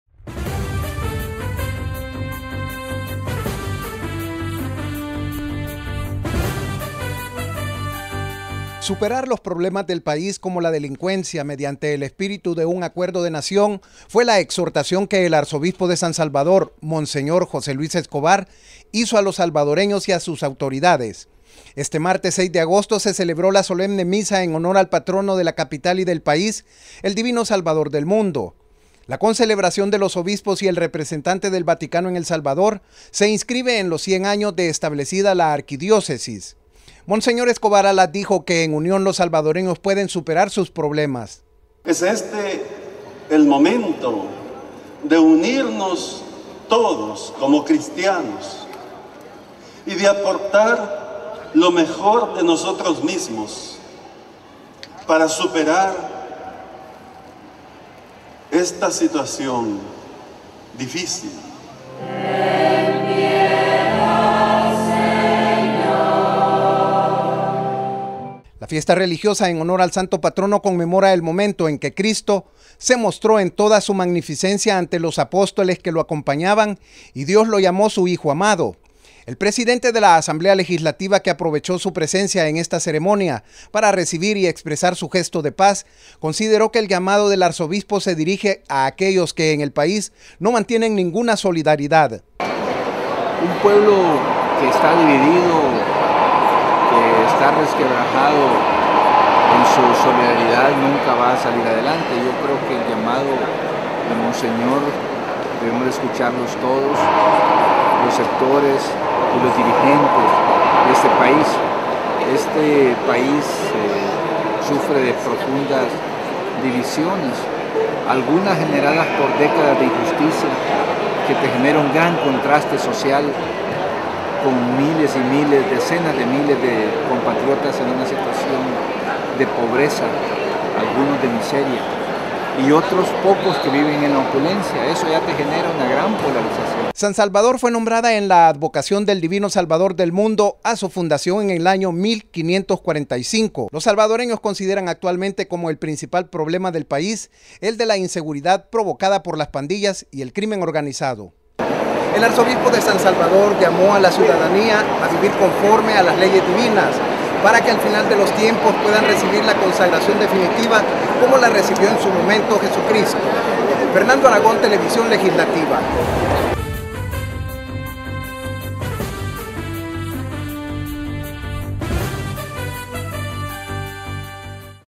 SANTO PATRONO
El presidente de la Asamblea Legislativa, Sigfrido Reyes, y el Arzobispo de San Salvador, Monseñor José Luis Escobar, coincidieron en que para superar los problemas del país, se requiere una dosis de solidaridad entre los ciudadanos, principalmente desde aquellos que tienen mucha riqueza.